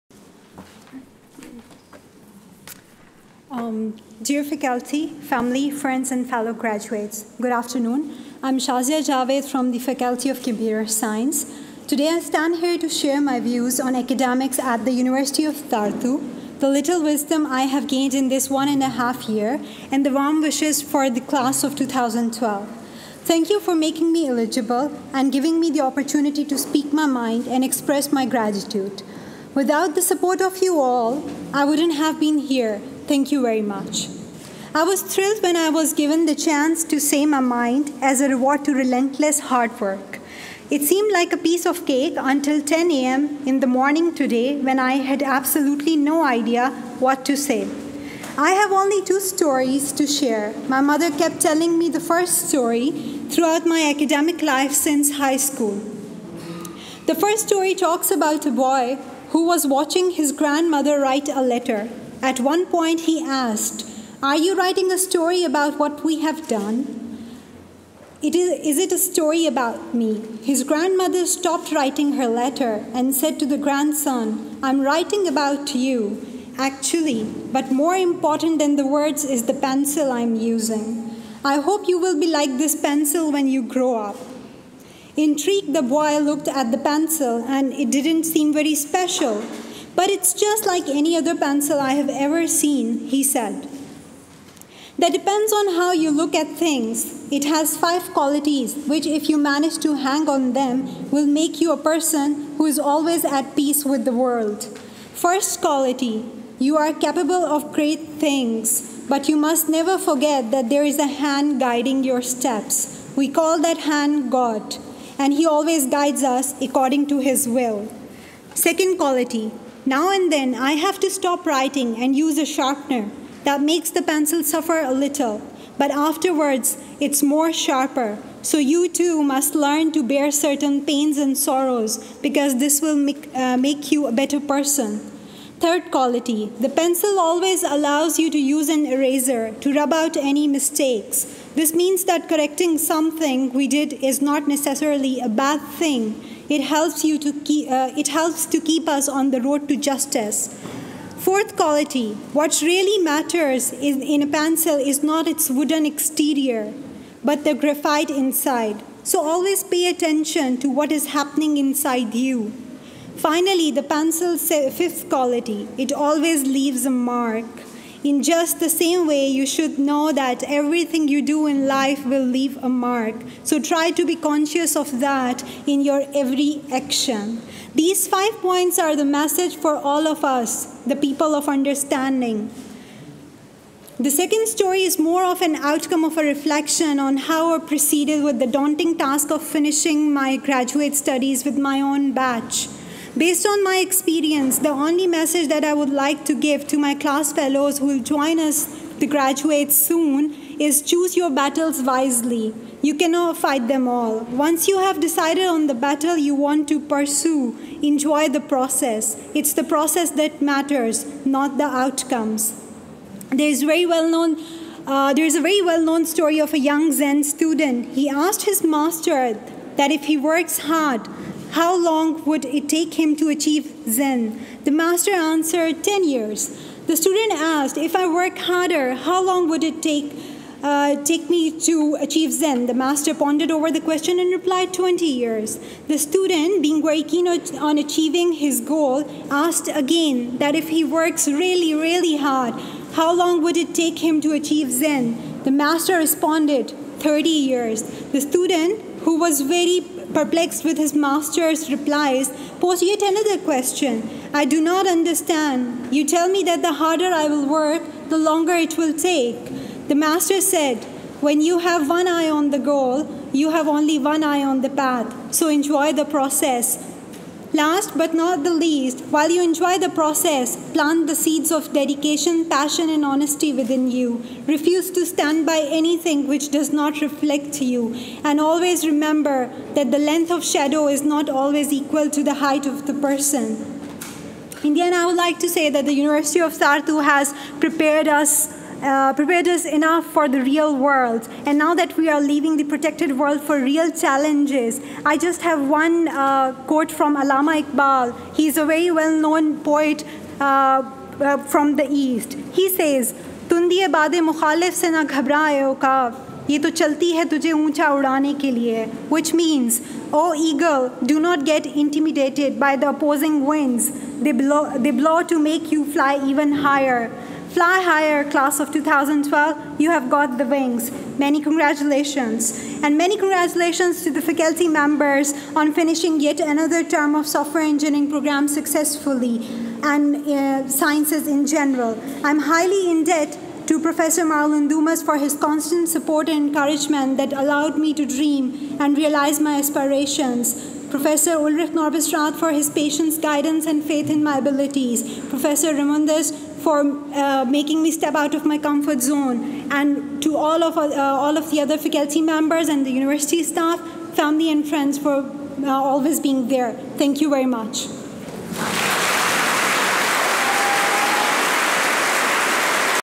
Shazia Javed's Speech at UT Graduation Ceremony
Shazia Javed, a cum laude Master's graduate in Software engineering, gives a speech at the graduation ceremony at the University of Tartu.